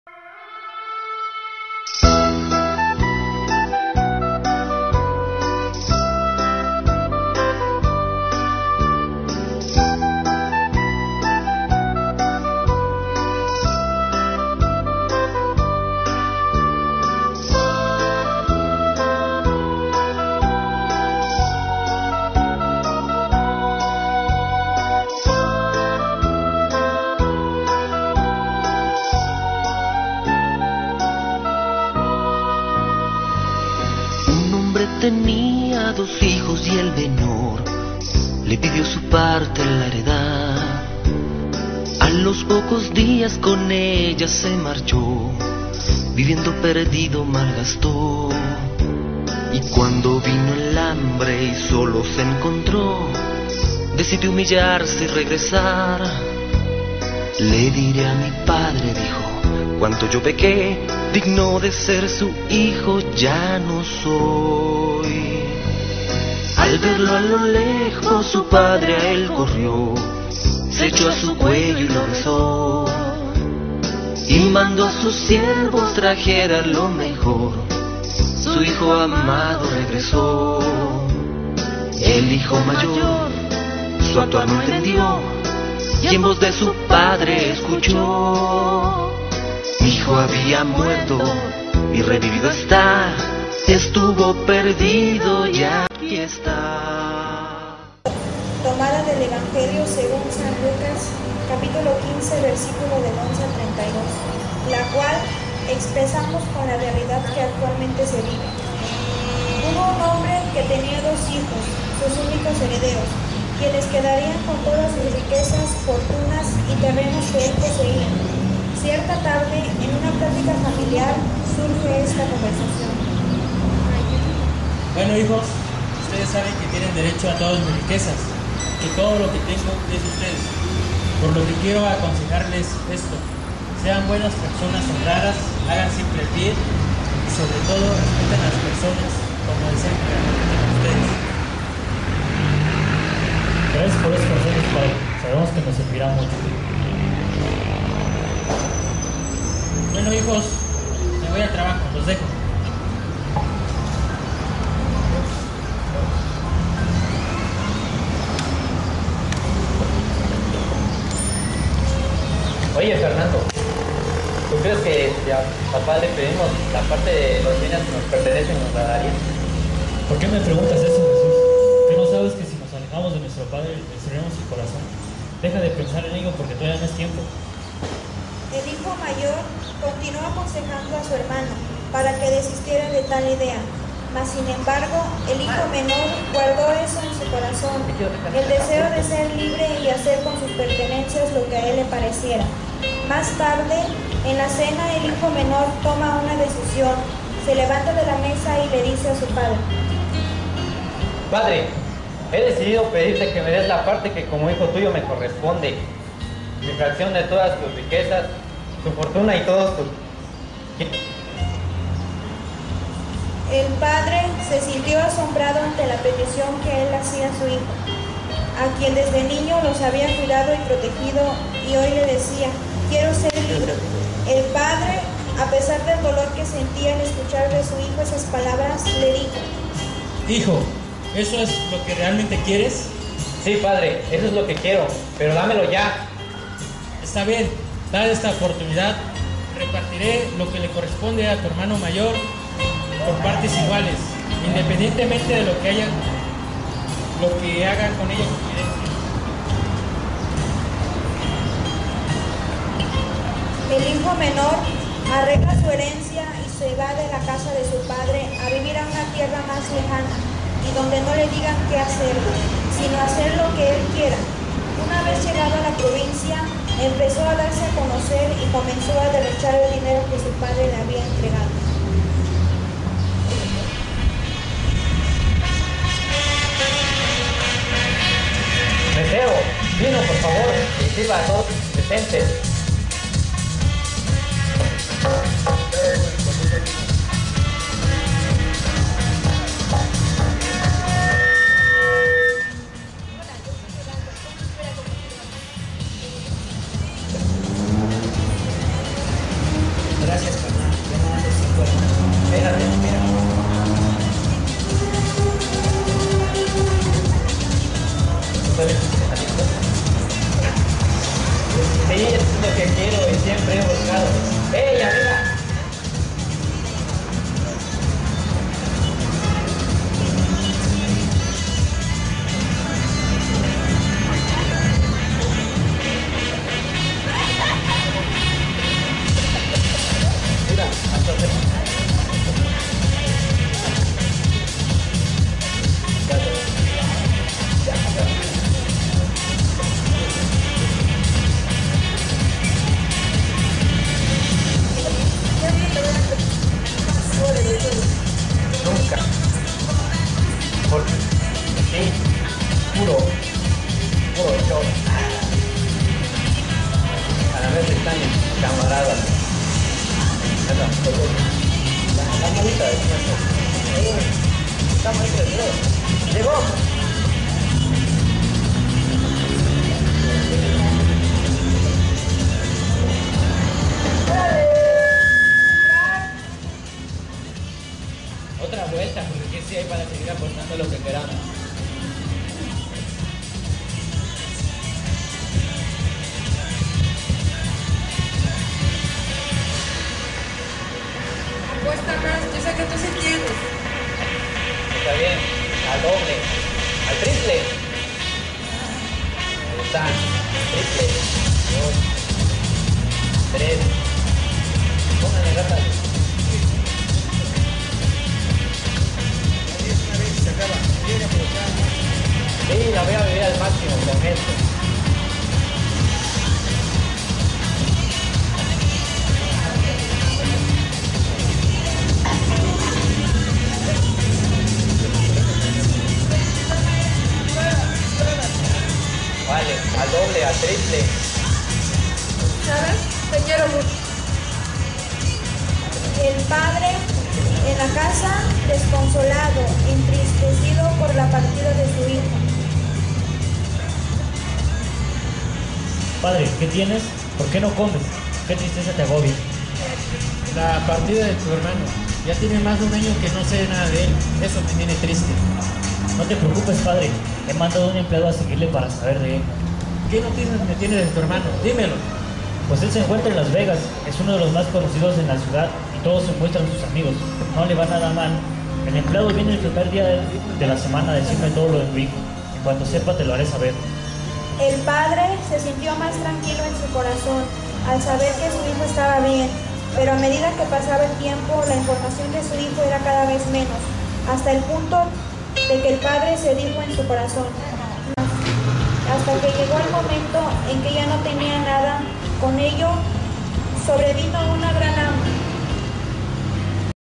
el hijo prodigo parte 1
El grupo de jóvenes  del Movimiento de Renovación Carismática Católica en el Espíritu Santo de san antonino castillo velasco,oaxaca tiene el privilegio de presentar a ustedes la parábola del "Hijo Prodigo" tomada del Evangelio según San Lucas 15, 11-32, la cual expresamos con la realidad que actualmente se vive.